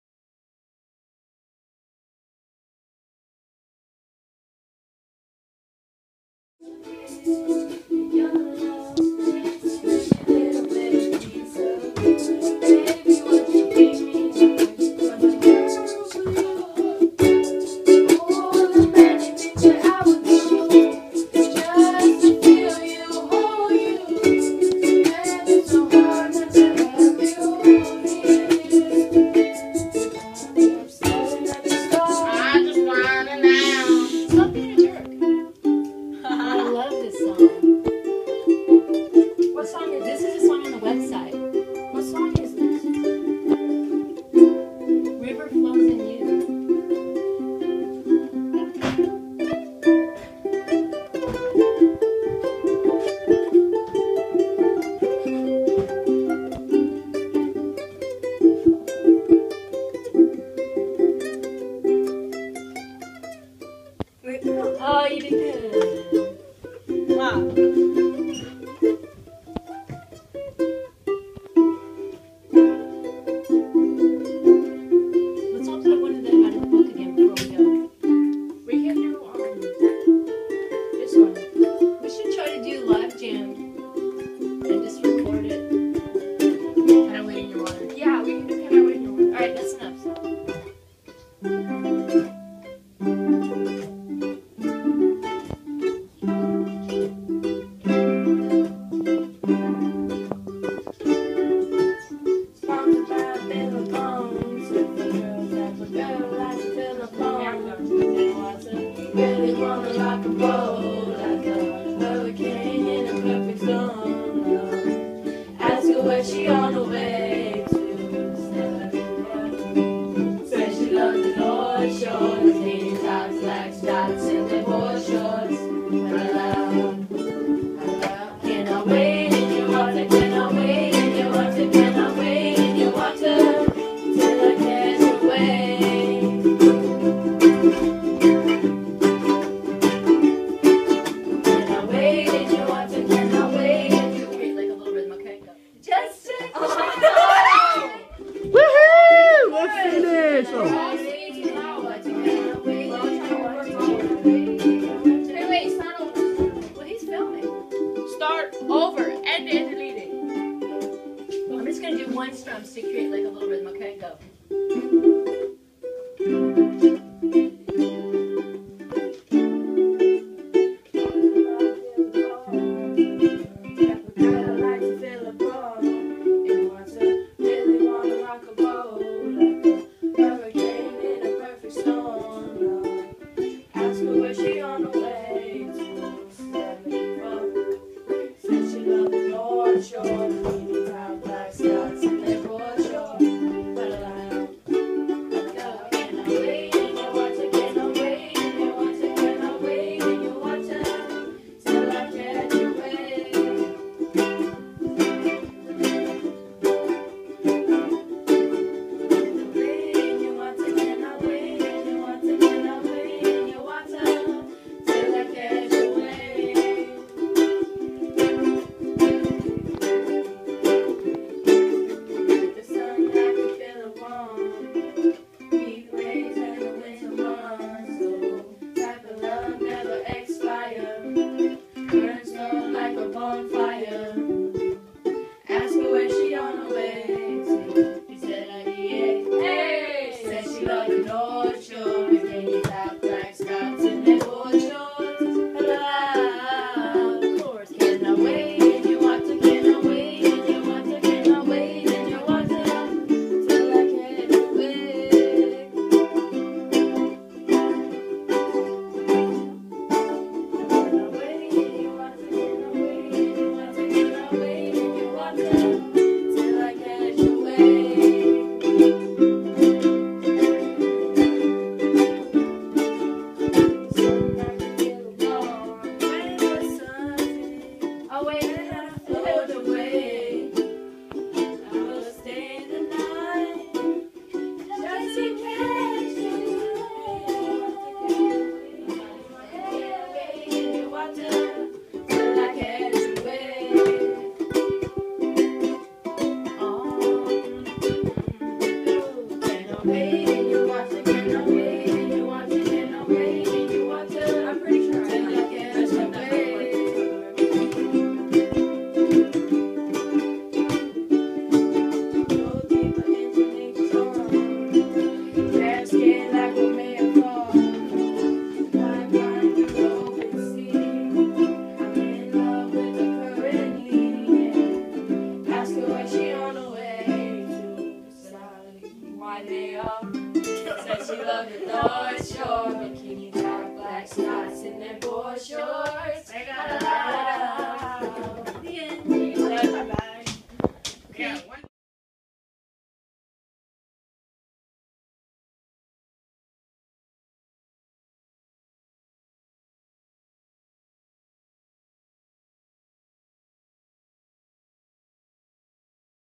Leilehua High School. Ukulele Class ♪ (5th) Period. Spring 2015
Students Jam. Leilehua High School Ukulele Class (5th) Period. Spring 2015  ♪ ♫ ♪